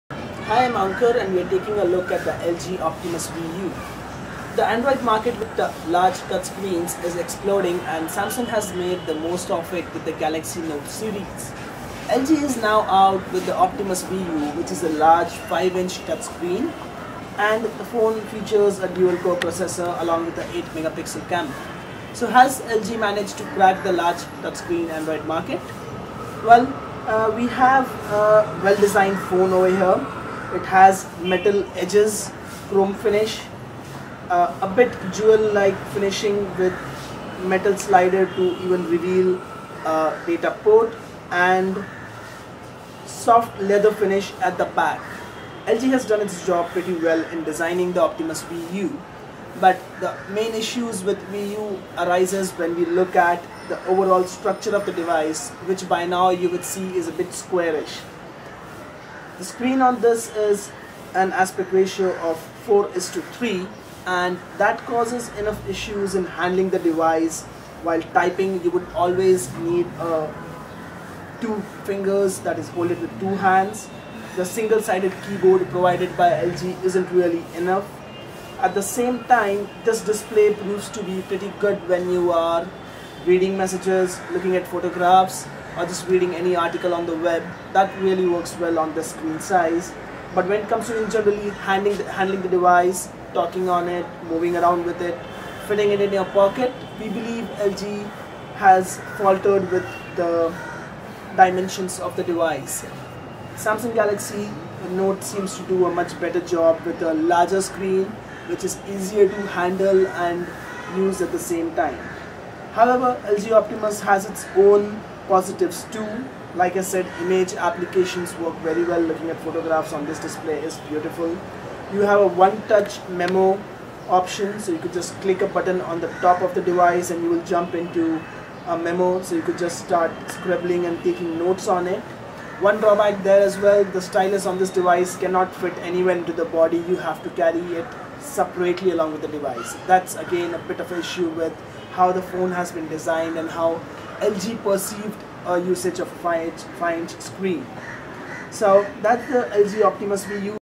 LG Optimus VU Quick Review
LG is out to compete with their rival Samsung and their Galaxy Note series. They released the LG Optimus VU, a large 5.0 inch touchscreen with 4:3 aspect ratio and a stylus, but did they really faltered with the form factor and made it even more difficult to use? Lets find out...